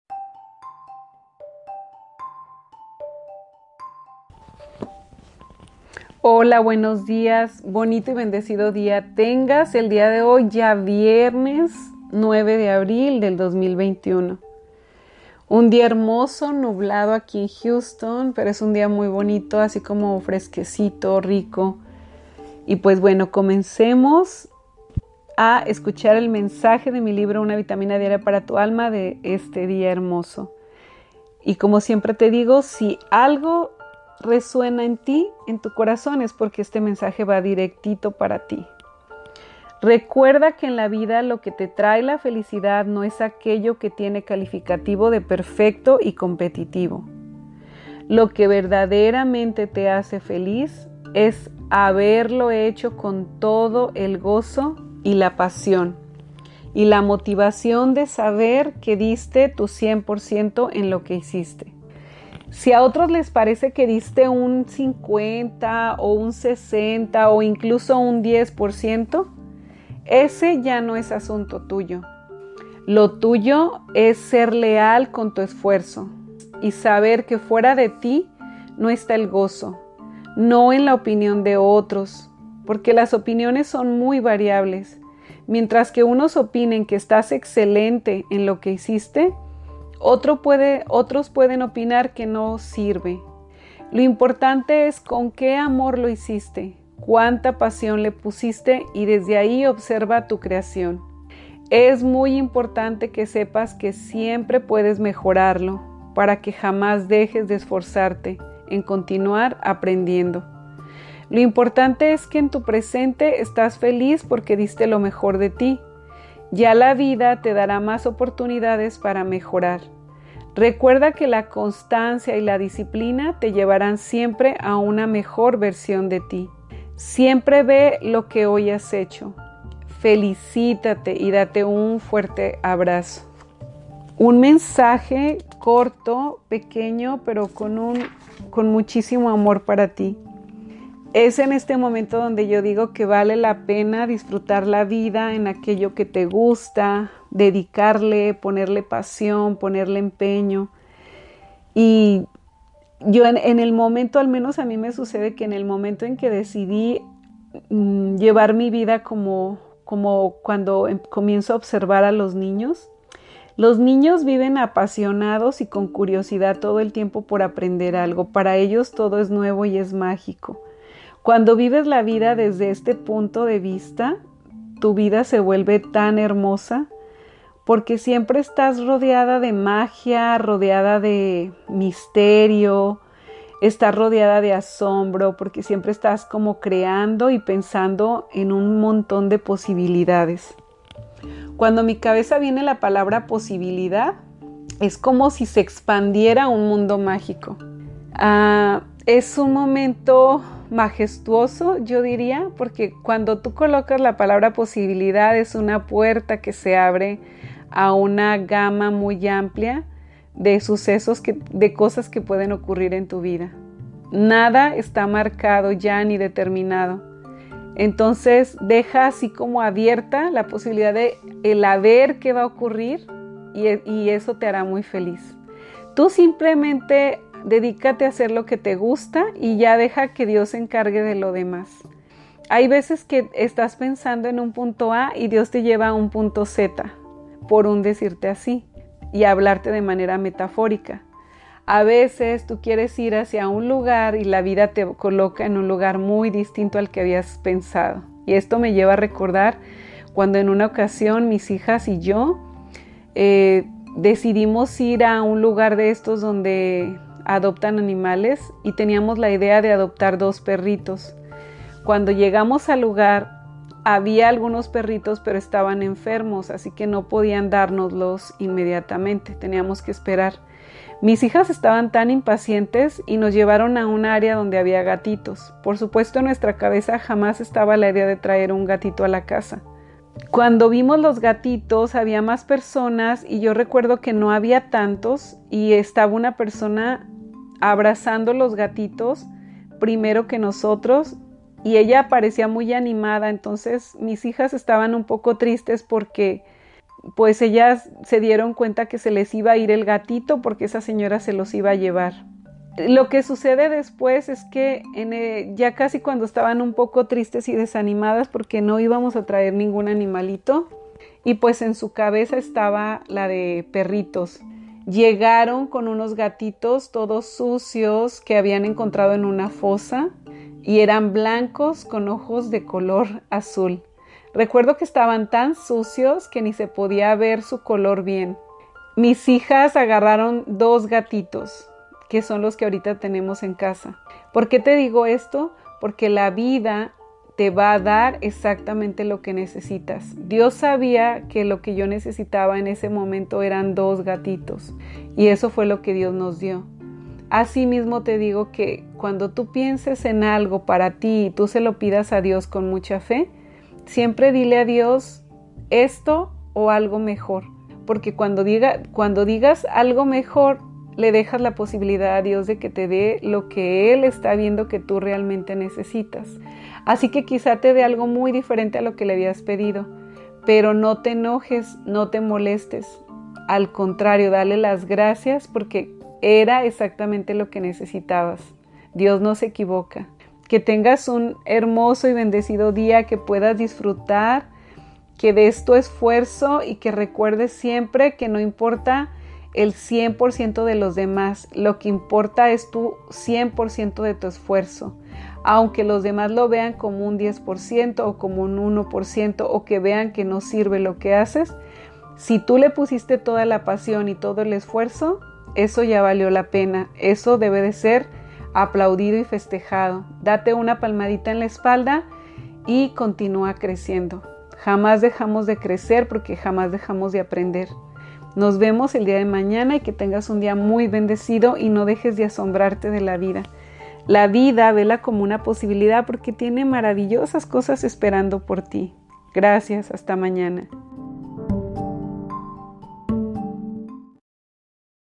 Podcast Autoconocimiento Letty López. Abril_9_2021wav.m4a, UNA VITAMINA DIARIA PARA TU ALMA.
Lo que verdaderamente te hace feliz es haberlo hecho con todo el gozo y la pasión y la motivación de saber que diste tú 100% en lo que hiciste, si a otros les parece que diste un 50% o 60% o 10%, ese ya no es asunto tuyo, lo tuyo es ser leal con tu esfuerzo y saber que fuera de Ti no está el gozo, no en la opinión de otros, porque las opiniones son muy variables, mientras que unos opinen que esta excelente lo que hiciste , otros pueden opinar que no sirve, lo importante es con que amor lo hiciste, cuanta pasión le pusiste y desde ahí observa tu creación, es muy importante que sepas que siempre puedes mejorarlo para que jamás dejes de esforzarte en continuar aprendiendo, lo importante es que en tu presente estás feliz porque diste lo mejor de Ti, ya la vida te dará más oportunidades para mejorar, recuerda que la constancia y la disciplina te llevarán siempre a una mejor versión de Ti. Siempre ve lo que hoy has hecho, felicítate y date un fuerte abrazo. Amado Ser de Amor.